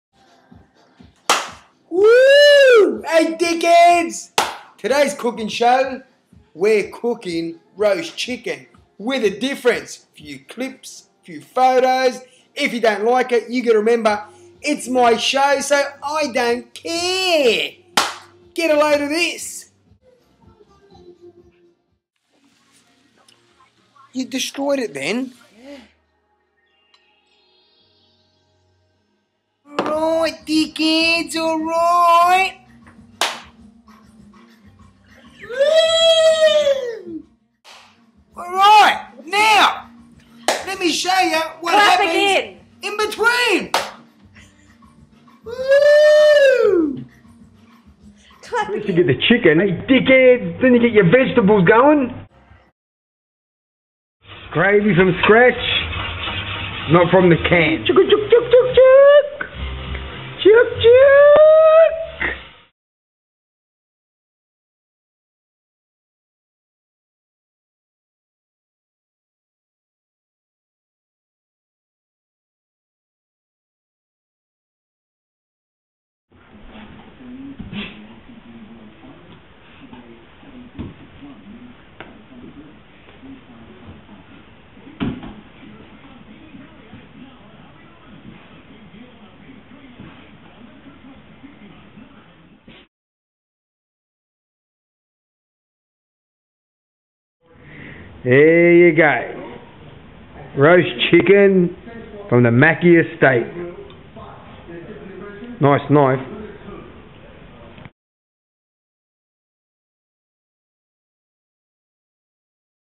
maky mumbanger making roast chicken COOKING SHOW
the world famous maky mumbanger making roast chicken..
this cooking show maky mumbanger  cooks and show you some personal videos and photos..
this masterchef is an incredible entertainer and a very special talent.